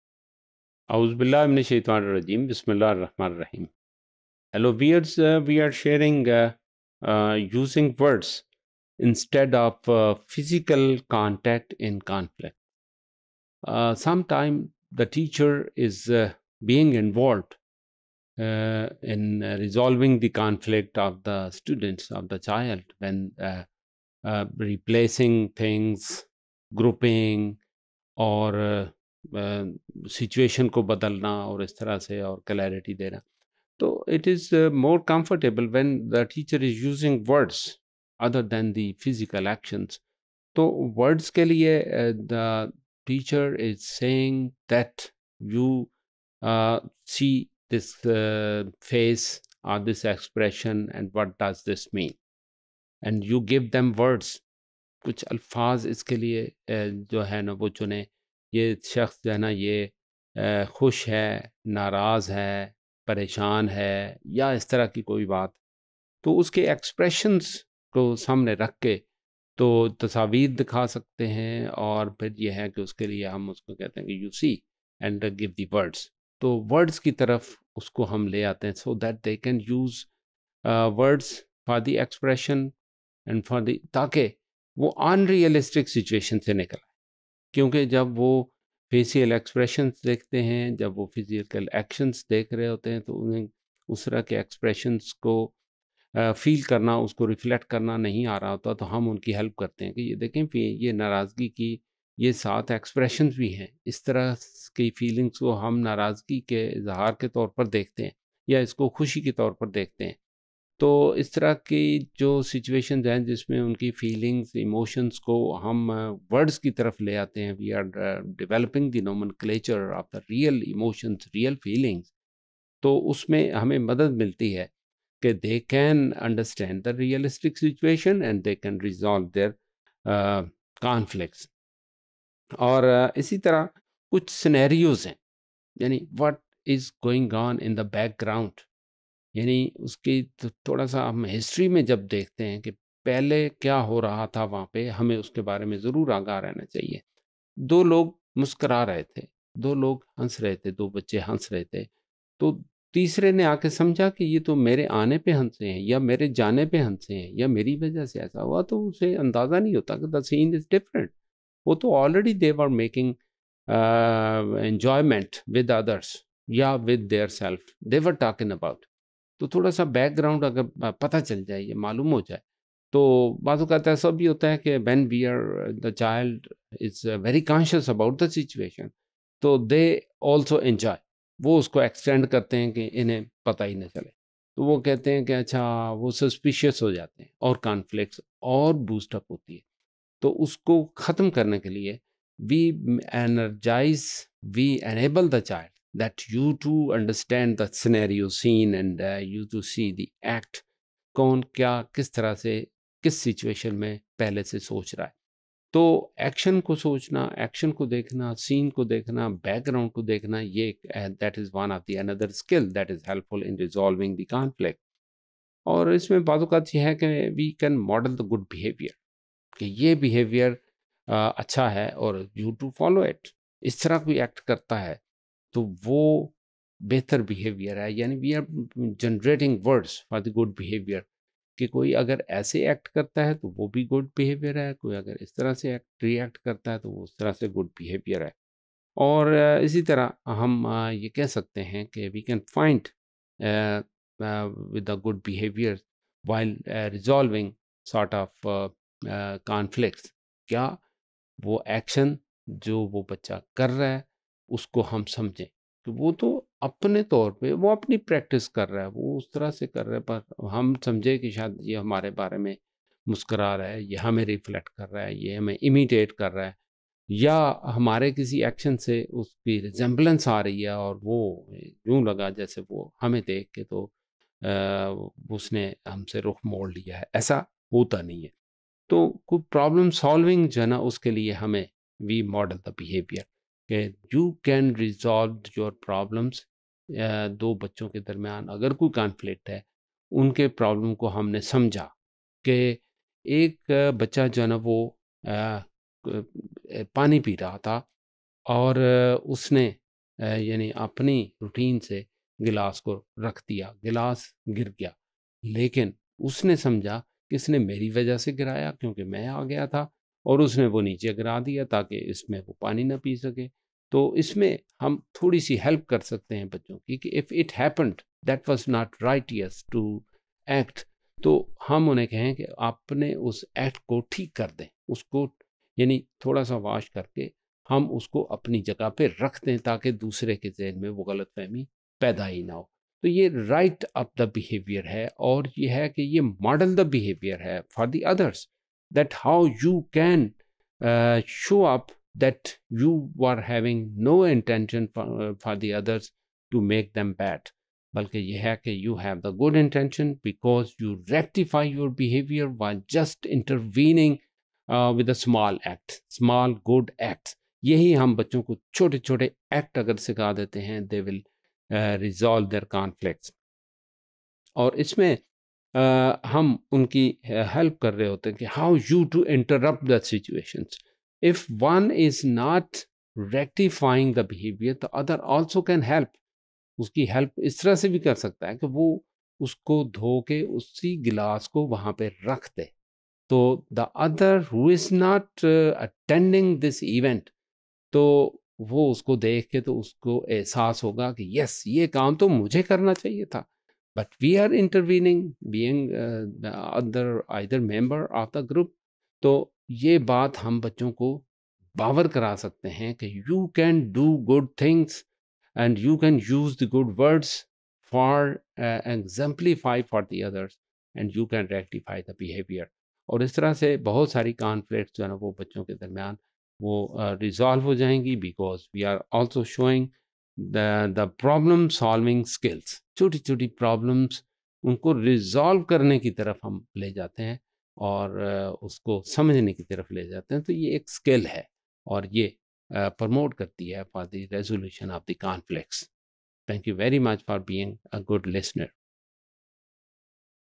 Using Words | Independence, Self-Management, and Ethics in Early Years | ECE204_Topic181
Topic181 - Using Words Instead of Physical Contact in Conflicts,